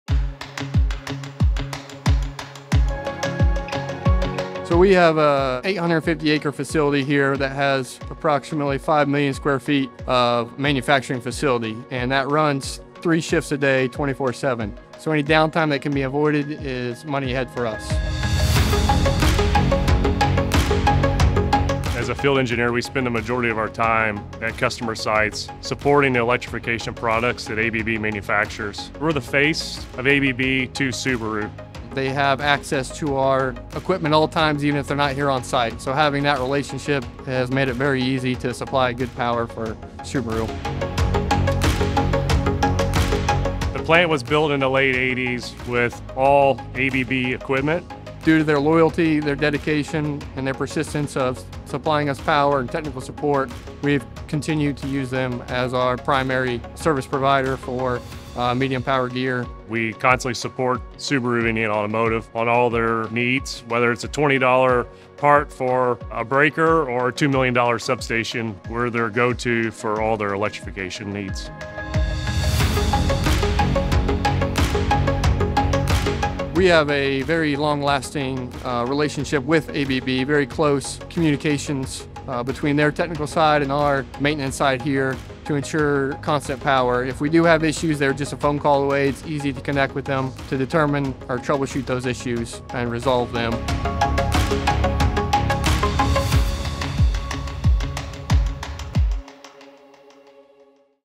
Keeping critical power on 24/7 at Subaru of Indiana Automotive - ABB Electrification Service
To stay ahead of the industry’s evolving needs and keep things running smoothly, Subaru of Indiana Automotive (SIA) needs access to safe, efficient electrical infrastructure and round-the-clock power. This is where ABB Electrification Service steps in. Having partnered with SIA for more than 30 years, our dedicated service team is phone call away to troubleshoot issues and make sure operations run smoothly.